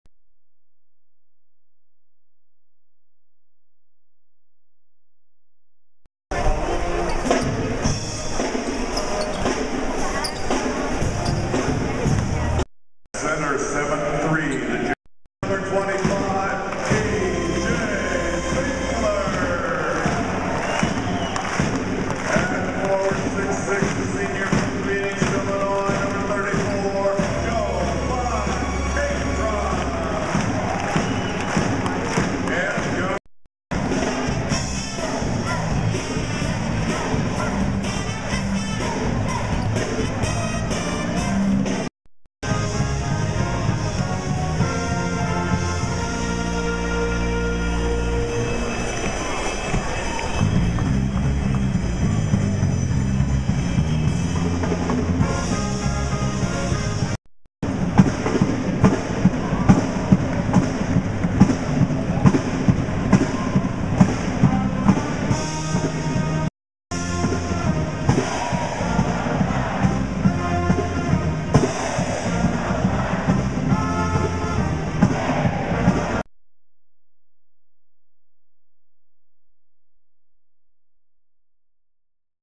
Oregon cheerleaders, 11-14-10.mov
Oregon cheerleaders in UCSB vs. UO on 11/14/10